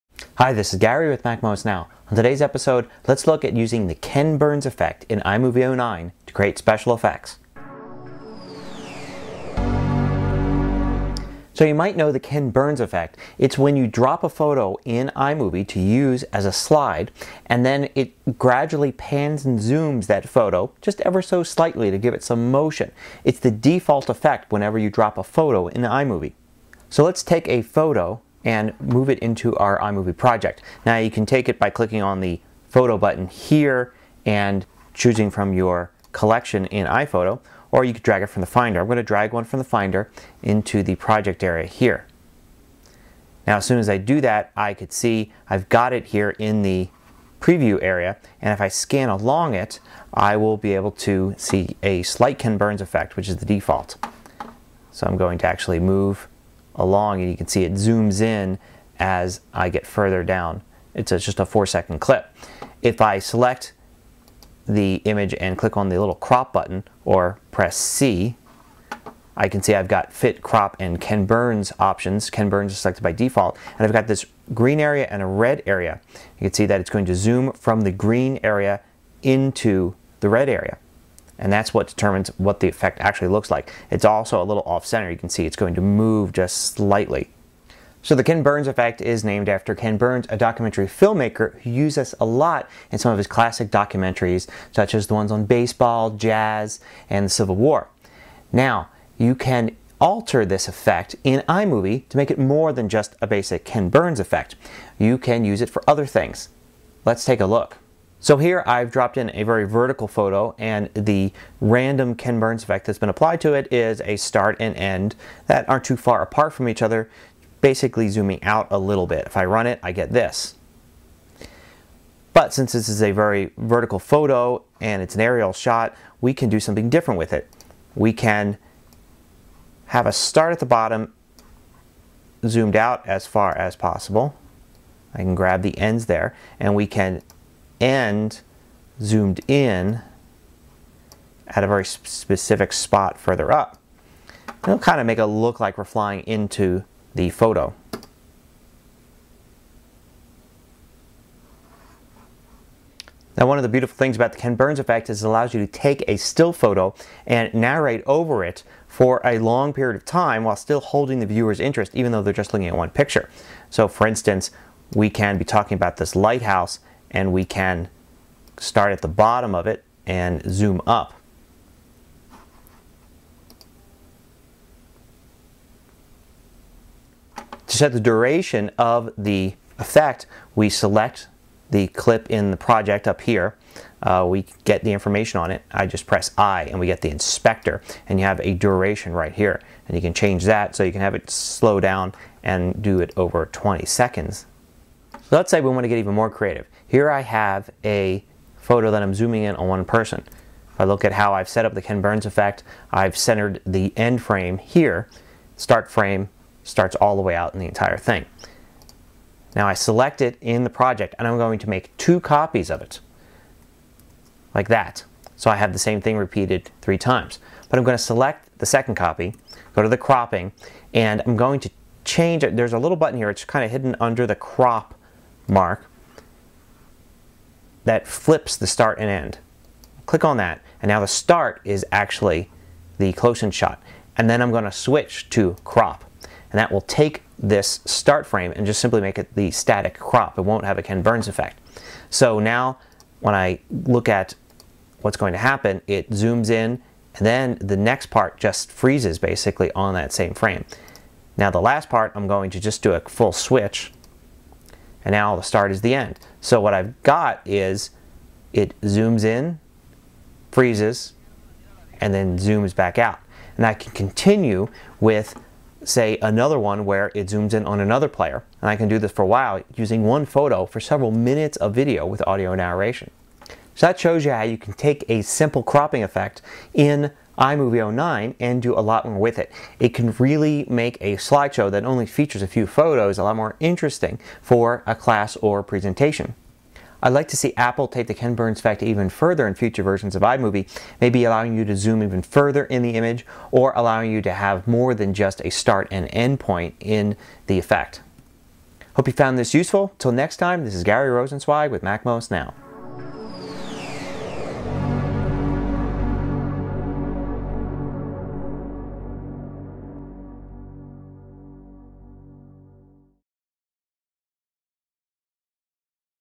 Beyond the Ken Burns Effect (MacMost Now 341)
The Ken Burns Effect in iMovie 09 is the default setting for photos inserted into an iMovie project. But you can use it for more than just adding a little motion. You can pan and zoom to focus on objects or move along an image. You can even zoom in and then back out and other similar effects.